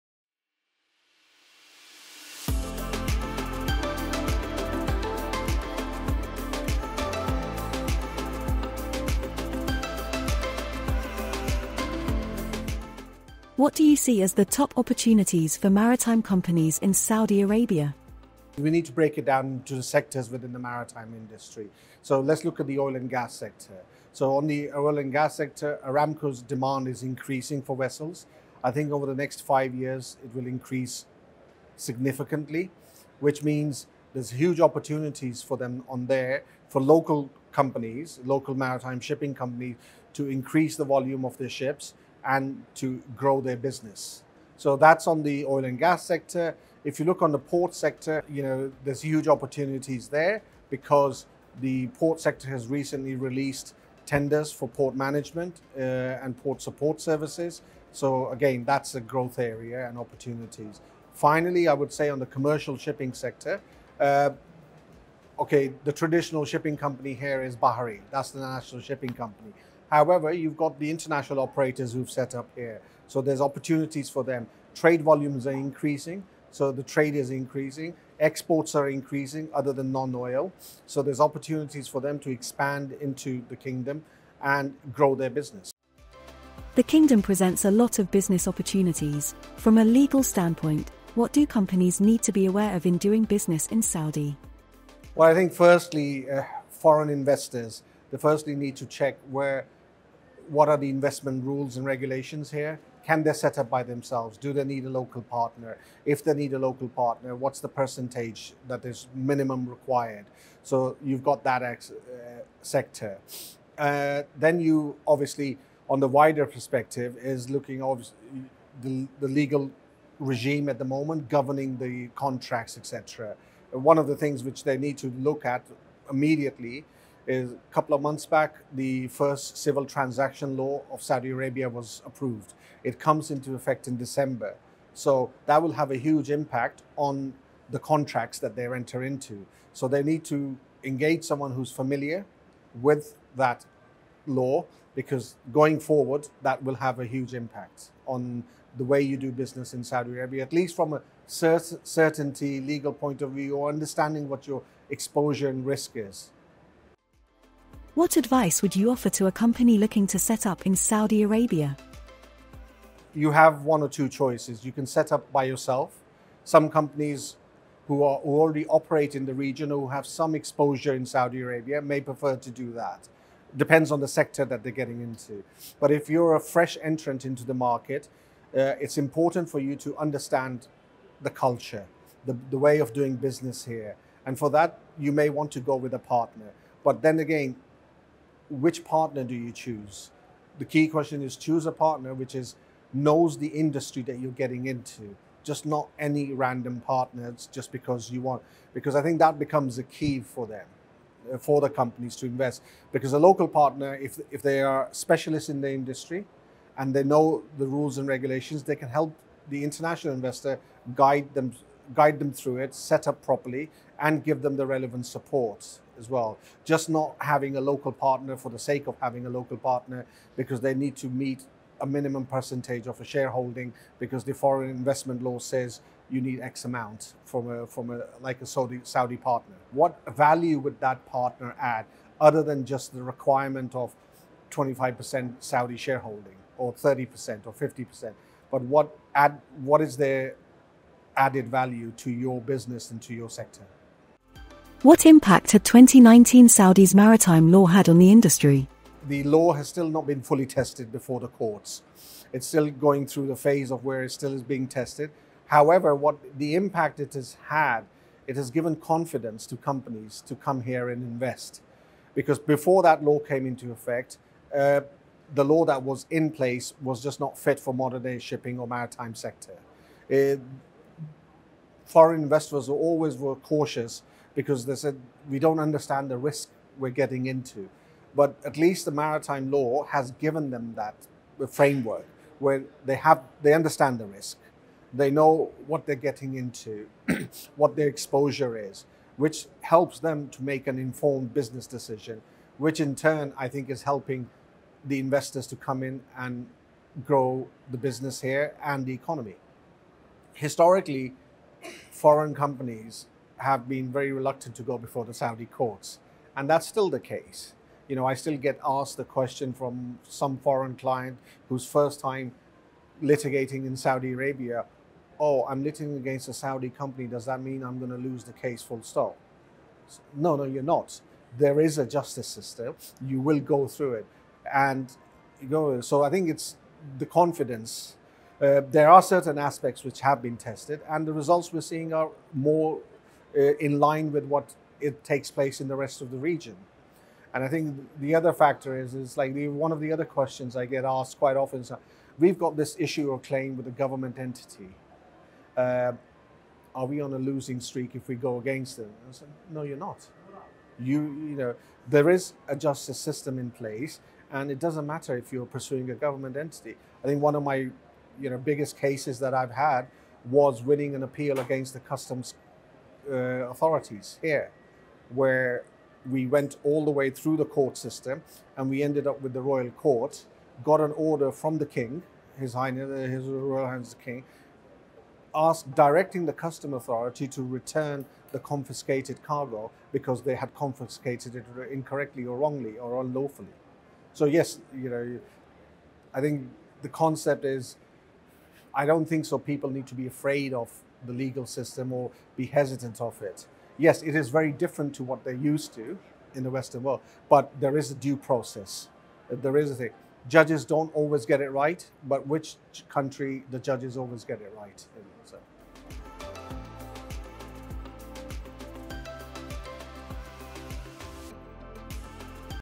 Legal considerations for maritime companies in Saudi Arabia
In this interview with Seatrade Maritime, Khurram Ali, Partner at Ince & Co Middle East LLP, discusses the top opportunities for maritime companies in Saudi Arabia, as well as the legal considerations that companies should be aware of when doing business in the Kingdom.

Khurram advises foreign investors to carefully review the Saudi investment rules and regulations, as well as the Kingdom's new civil transaction law, which comes into effect in December 2023. He also recommends that foreign investors consider partnering with a local company that has expertise in the maritimeindustry and can help them navigate the legal system in Saudi.

Learn more about the top opportunities and legal considerations for maritime companies in Saudi Arabia by watching the full interview.

Timestamps
00:00:00 Intro
00:00:16 What do you see as the top opportunities for maritime companies in Saudi Arabia?
00:01:28 The Kingdom presents a lot of business opportunities – from a legal standpoint what do companies need to be aware of in doing business in Saudi?
00:02:37 What advice would you offer to a company looking to set up in Saudi Arabia? For example, choosing a local partner etc.
00:04:05 What impact had 2019 Saudi’s maritime law had on the industry?